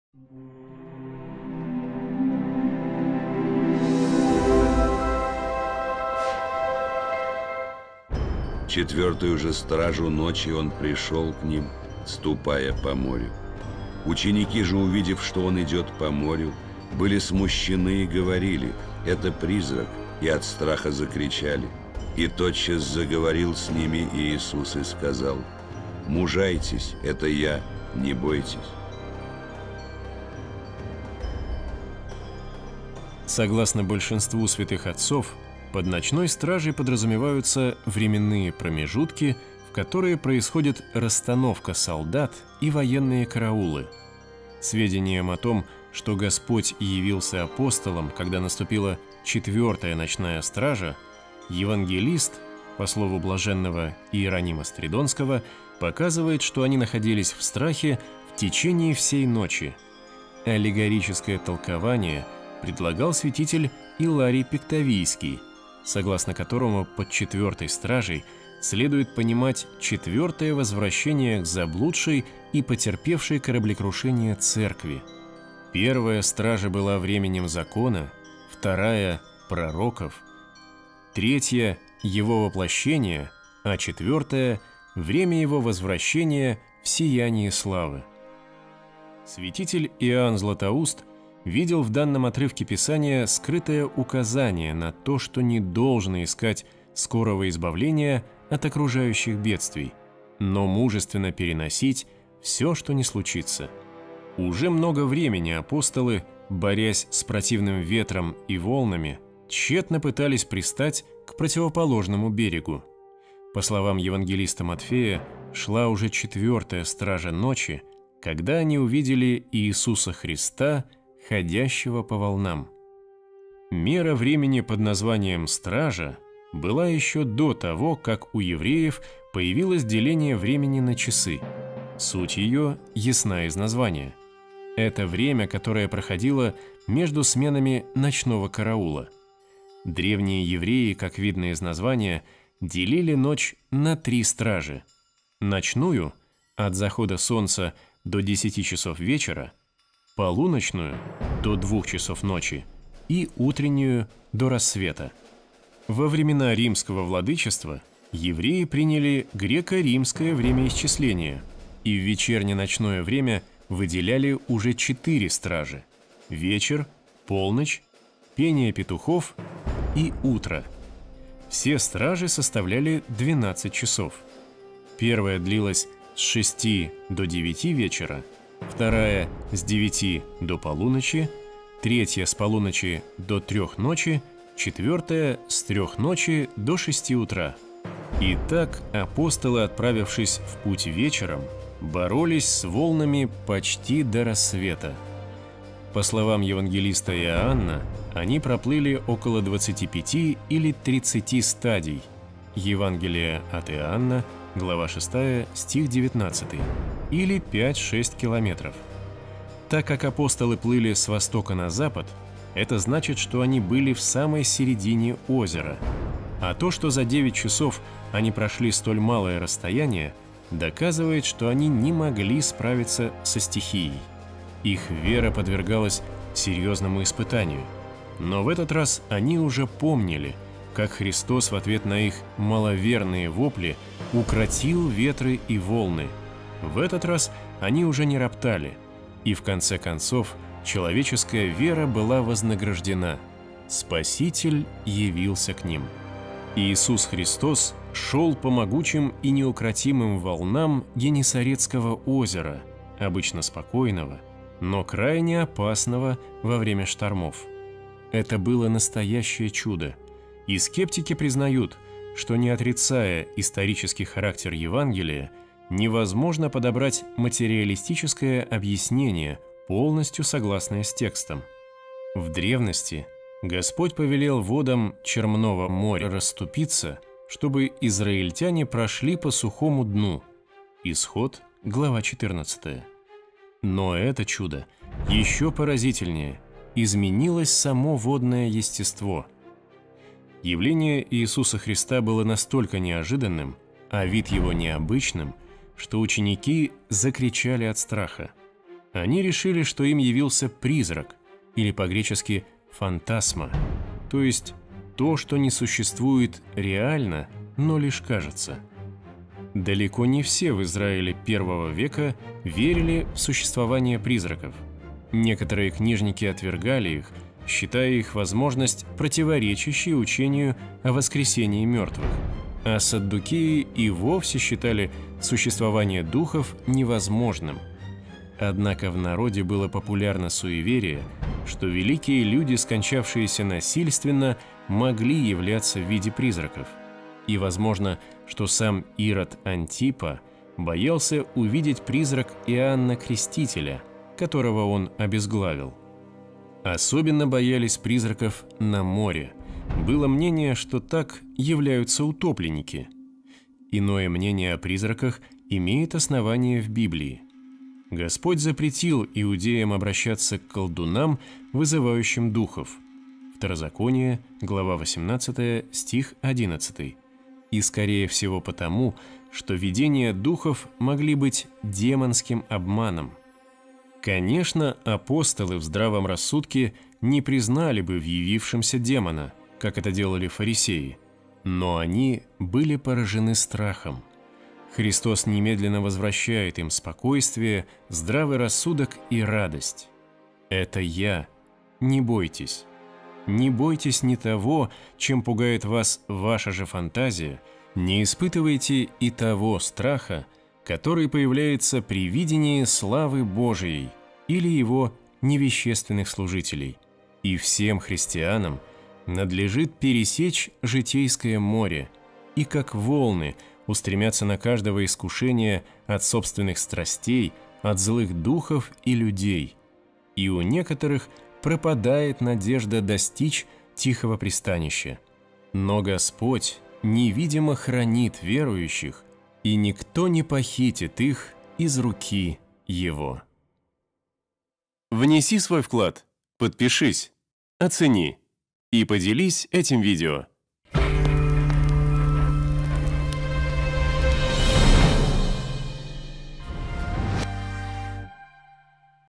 Как Христа перепутали с призраком? Толкования святых.
Ученики Христа спутали Его с призраком: святоотеческое и историко-культурное осмысление библейского сюжета.